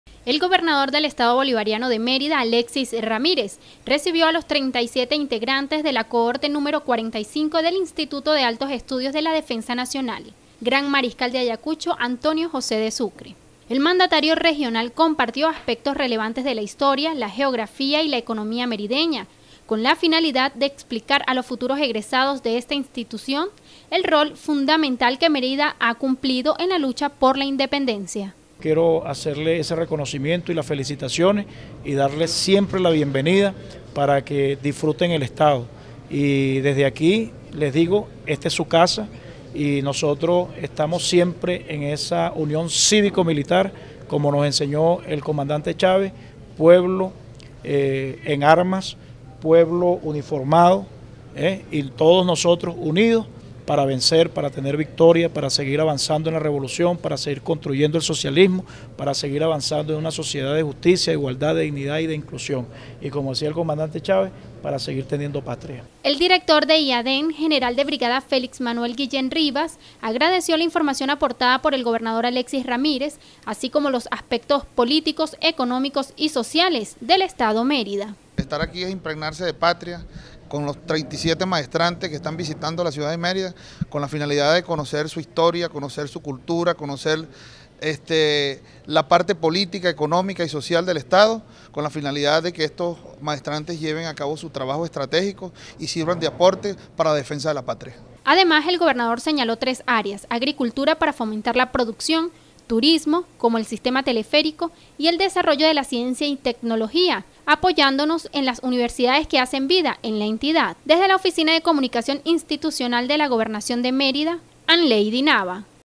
Alexis Ramírez recibió a cohorte 45 del Instituto de Altos Estudios de la Defensa Nacional
Unión cívico-militar

*** Durante su visita a la entidad los maestrantes conocerán la historia, así como los aspectos políticos y sociales del estado Mérida, para profundizar el plan estratégico para la defensa de la Patria.

Este martes el gobernador del estado Bolivariano de Mérida, Alexis Ramírez, recibió a los 37 integrantes de la cohorte número 45 del Instituto de Altos Estudios de la Defensa Nacional (Iaeden) “Gran Mariscal de Ayacucho Antonio José de Sucre”.

Durante el encuentro realizado en el Salón del Pueblo del Palacio de Gobierno, el mandatario estadal compartió con los presentes los aspectos más relevantes de la historia, la geografía y la economía merideña, con la finalidad de explicar a los futuros egresados de esta institución el rol fundamental que Mérida ha cumplido en la lucha por la independencia.

“Fue en Mérida donde por primera vez se le concedió a Simón Bolívar el título de Libertador y durante su Campaña Admirable del año 1813 nuestro estado aportó hombres, caballos y provisiones, muchas de ellas de manos de mujeres que pasaron a la historia como las heroínas merideñas”, dijo Ramírez.

En el aspecto geográfico el gobernador Alexis Ramírez aclaró que el estado posee todos los pisos térmicos, desde las zonas costeras del Sur del Lago hasta las montañas más altas en la Sierra Nevada, con diversas condiciones climáticas que favorecen el desarrollo de una gran diversidad de actividades turísticas y agrícolas.

“En el campo económico nosotros actualmente estamos trabajando en tres grandes áreas, comenzando con la agricultura, para continuar potenciando la producción en diferentes pisos climáticos; luego el turismo con obras emblemáticas como el nuevo Sistema Teleférico Mukumbarí; y en tercer lugar está el desarrollo de la ciencia y la tecnología, apoyándonos en las 14 universidades que hacen vida en la entidad”, dijo.

El director del Iaeden, General de Brigada Félix Manuel Guillén Rivas, agradeció la información aportada por el gobernador Alexis Ramírez a este grupo de maestrantes que están conociendo la historia, así como los aspectos políticos y sociales del estado Mérida,  con la finalidad de profundizar el plan estratégico para la defensa de la Patria.

 El gobernador Alexis Ramírez reiteró su voluntad de continuar fortaleciendo en la entidad la unión cívico-militar, para avanzar en los objetivos plasmados por el Comandante Chávez en el Plan de la Patria. (OCI/Alonso Moreno).